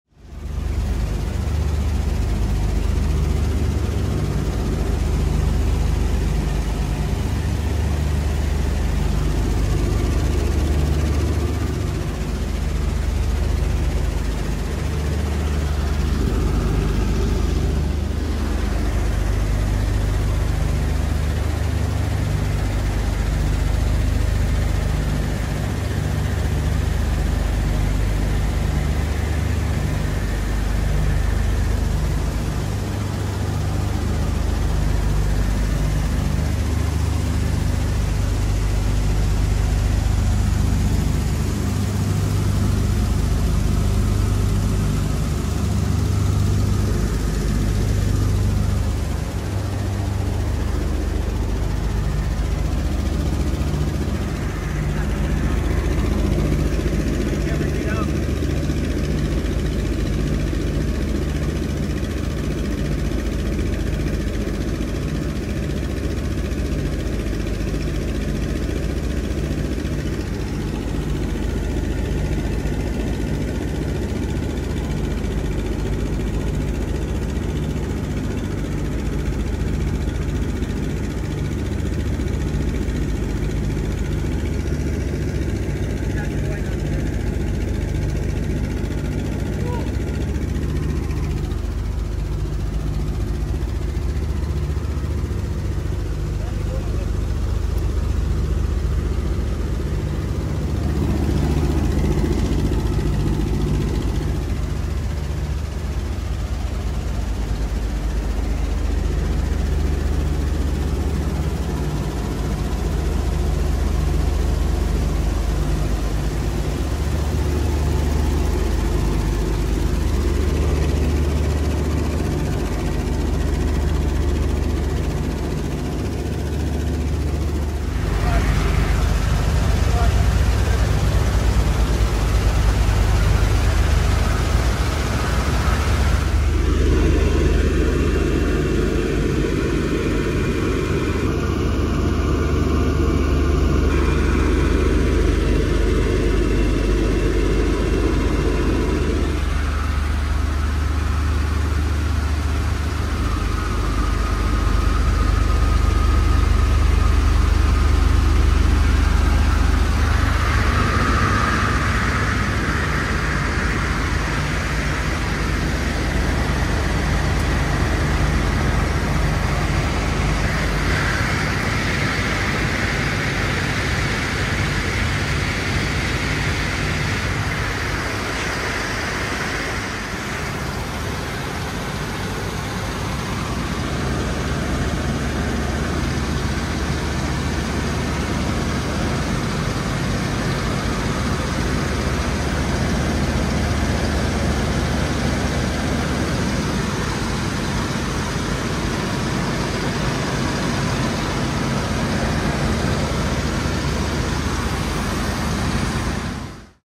Berlin Express Cockpit and Return to Waist Gun Position
Video from my flight aboard the B-25
'Berlin Express" on July 23, 2024 during EAA Airventure.
After vising the cockpit area I made my way back to the midsection of the aircraft.